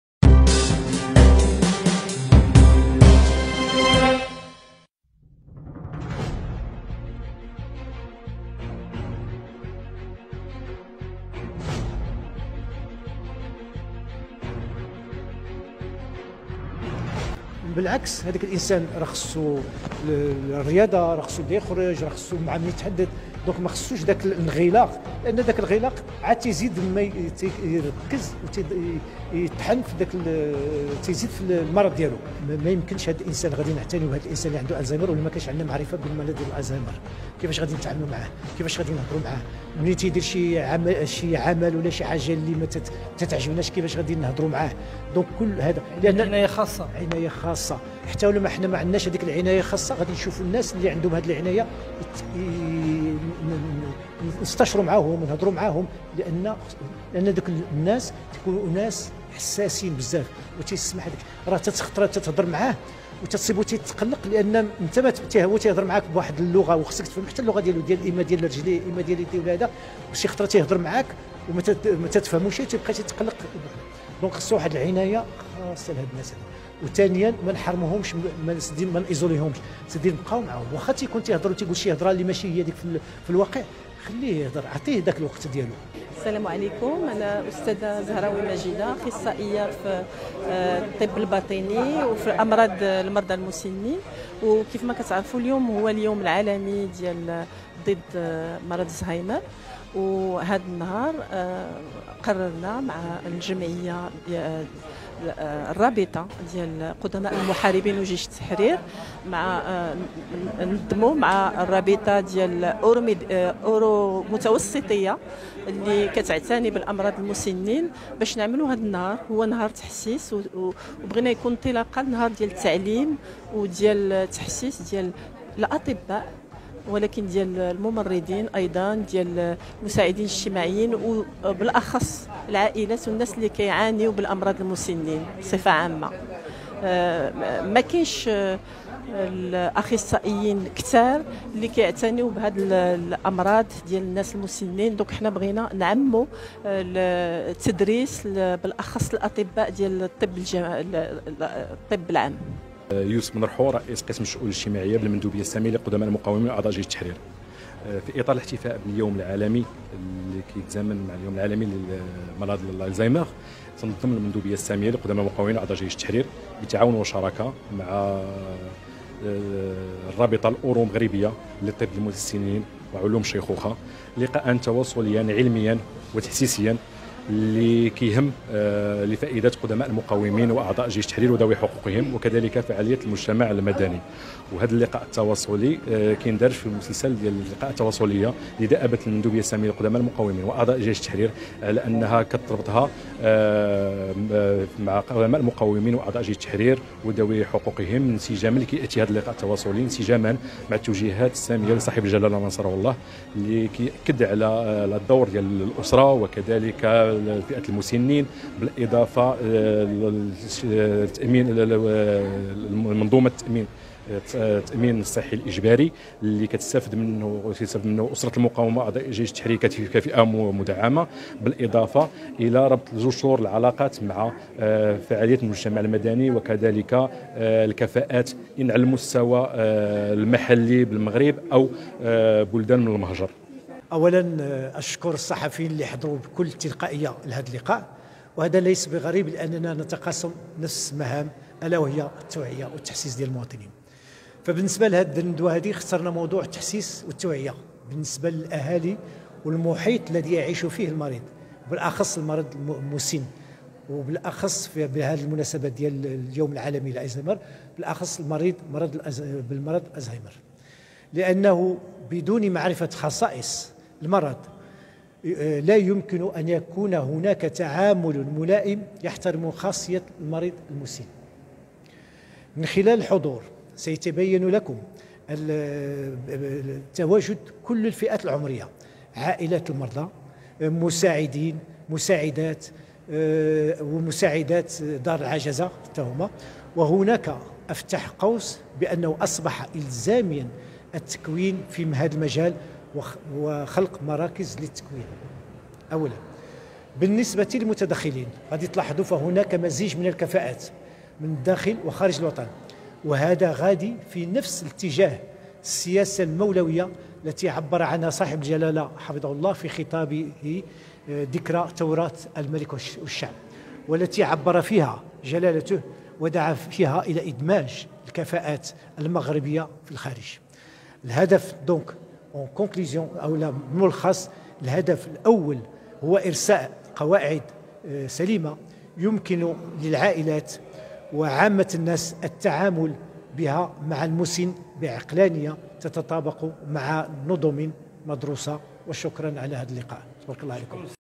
بمناسبة اليوم العالمي لمرض الزهايمر ... نصائح مهمه للقائمين على رعاية أقاربهم المرضى
متابعة وحوار الإعلامي
توضيب